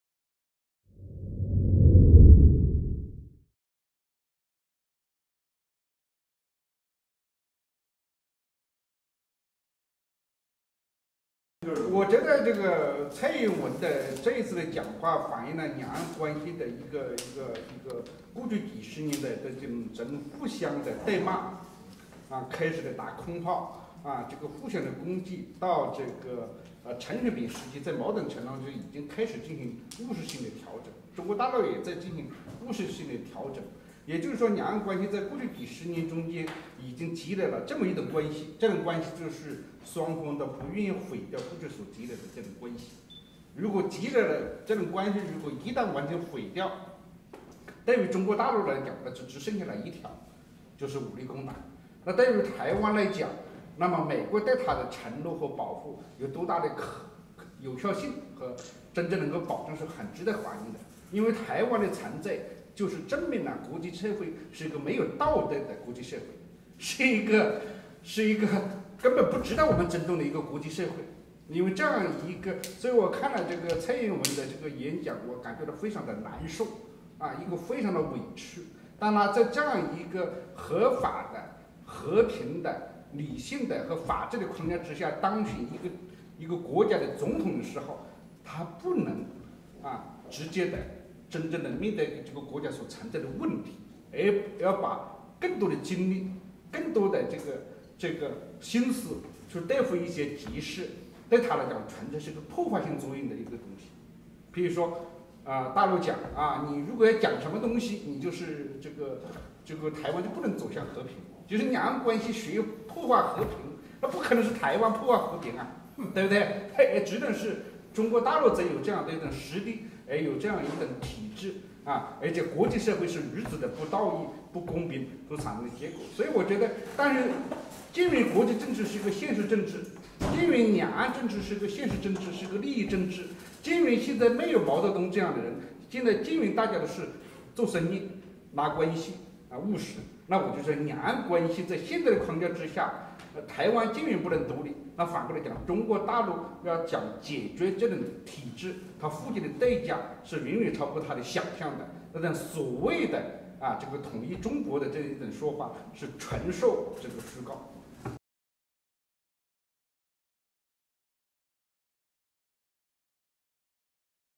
何頻：兩岸都在進行務實性的調整（中國研究院蔡英文就任後兩岸關係研討會）
2016年5月21日，中國研究院在美國紐約舉行了第31次研討會，討論蔡英文就任總統之後的兩岸關係。此視頻為明鏡新聞出版集團總裁何頻的發言。（明鏡電視）

中國研究院研討會合輯系列：
《紅色帝國》